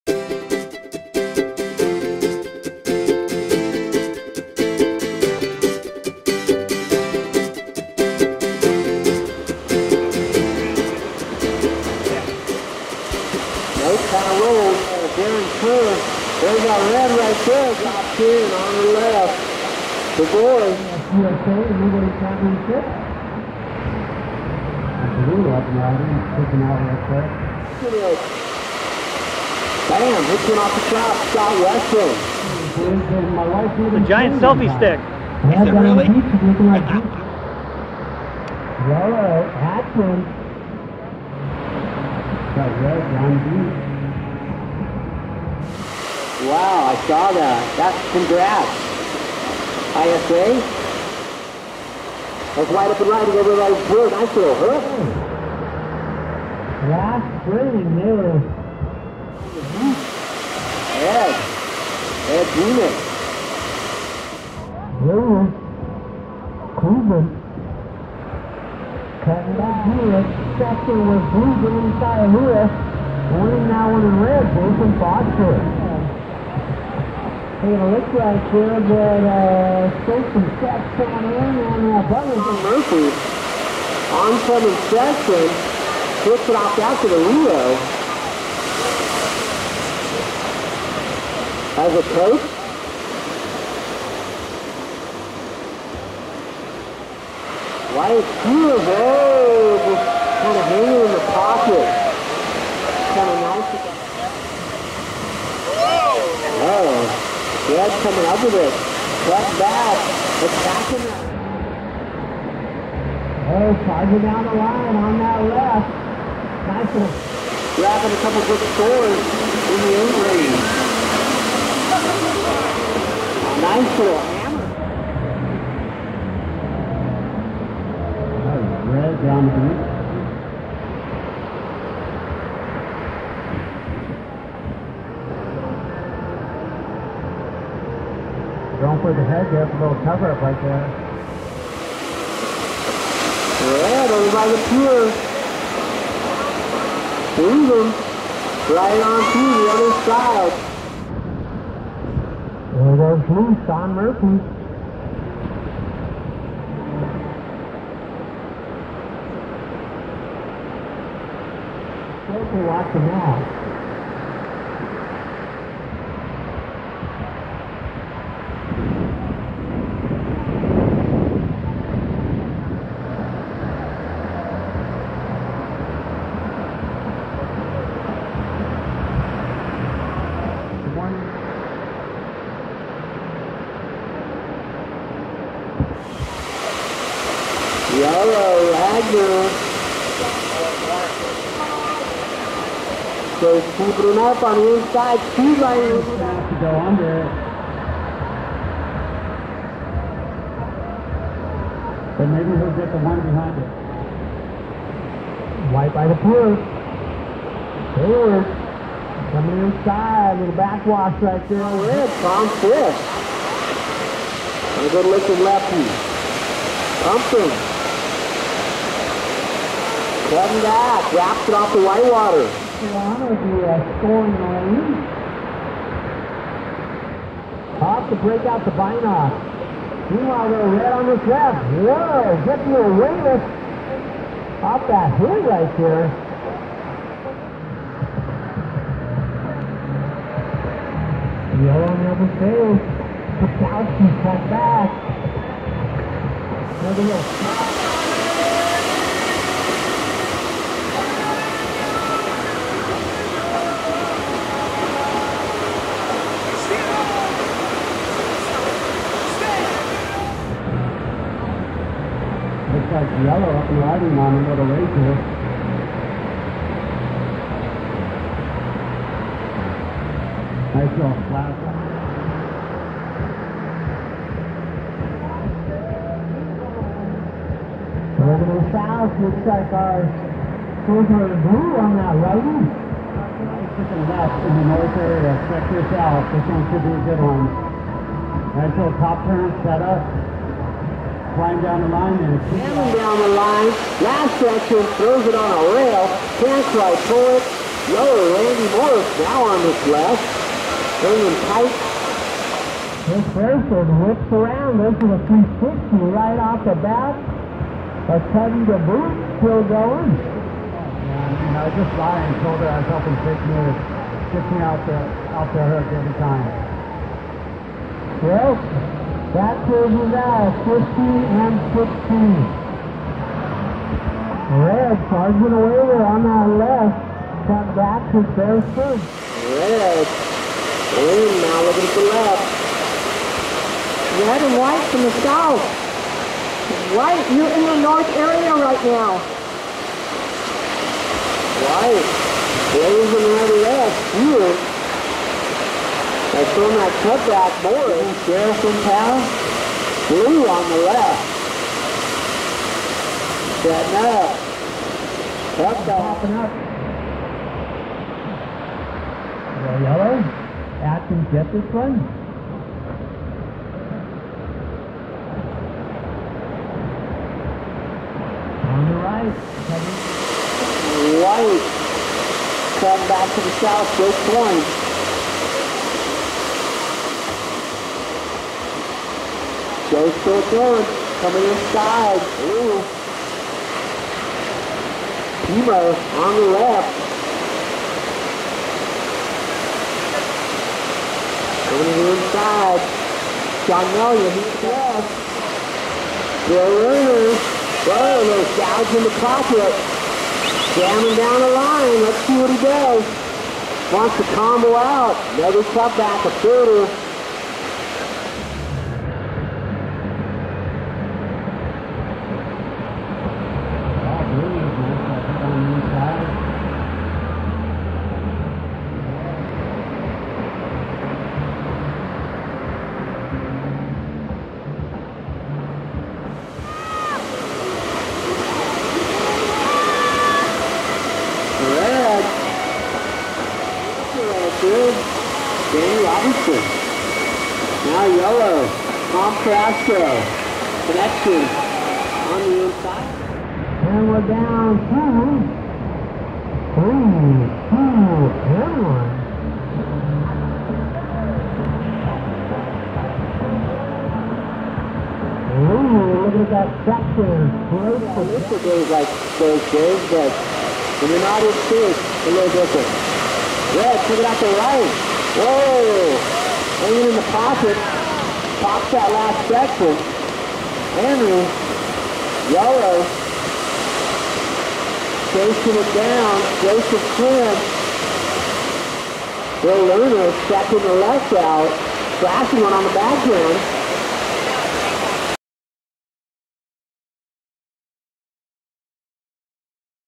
2018 US Titles Chapter Two (Morning Highlights) ksusa kneelo
2018 US Titles and Kneeboard Festival returns to Huntington Beach Pier. 62 top boarders gathered for their annual national contest. The surfing USA hosted one day event was held with 58 deg. water temp, light winds, and 2/3 ft surf and announced by the world famous 'Rock'n Fig.